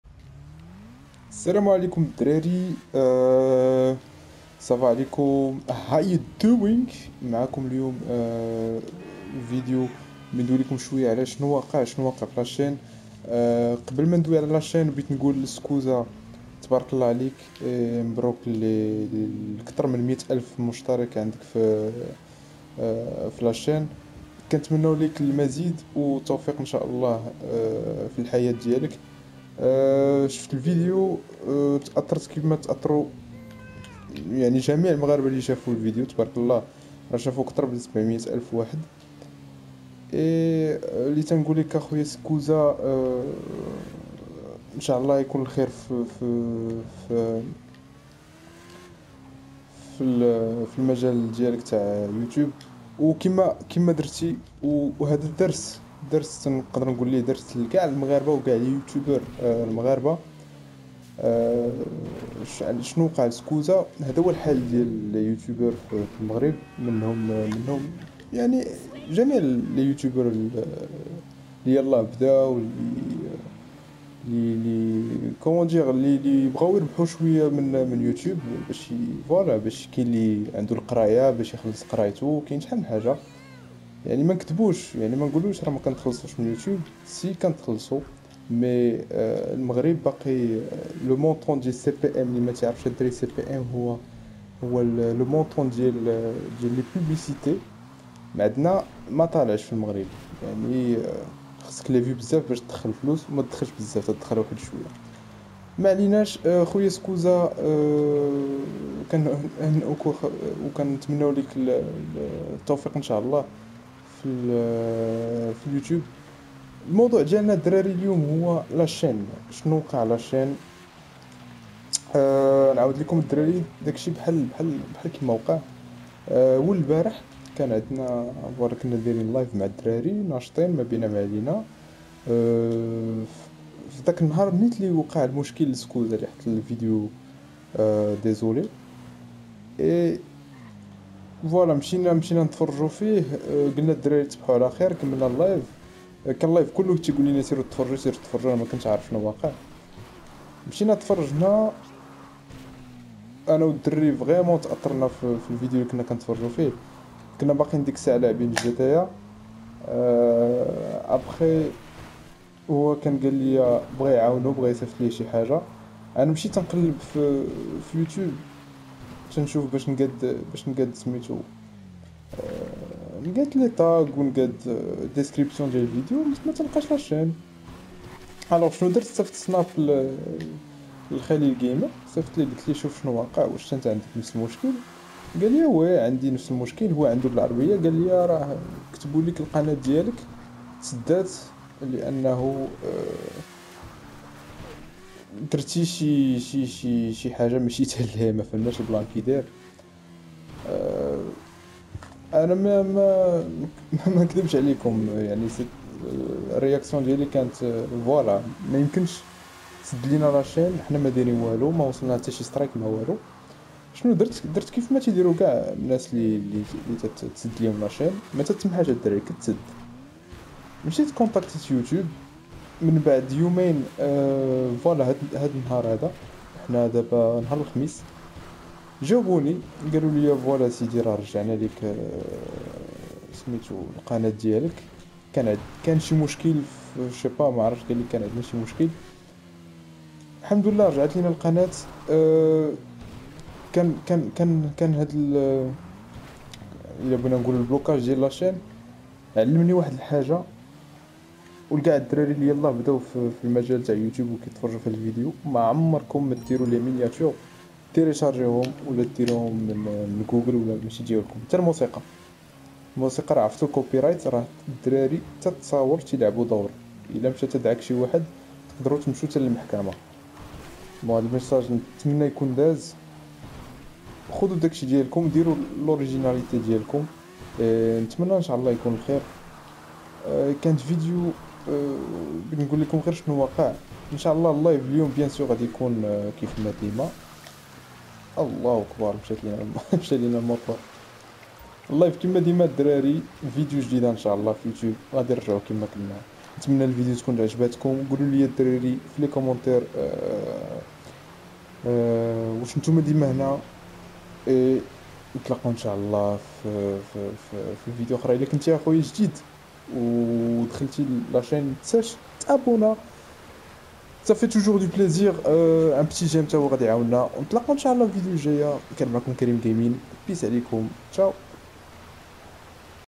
MBROUK 100K SKOWZA - KIFACH MSHAT LIYA LA CHAINE كفاش مشات ليا القناة
hamdoulah rj3at lina la chaine , choukran lga3 li 3awnouni :)
Rendez vous koula nhar a drari lvideo jdida , w m3a 22h live :)

un petit pouce bleu pour plus de vidéos

Si vous êtes la , sachez que vous êtes des pro gamers :)
Karim .
PSN : Karimgaming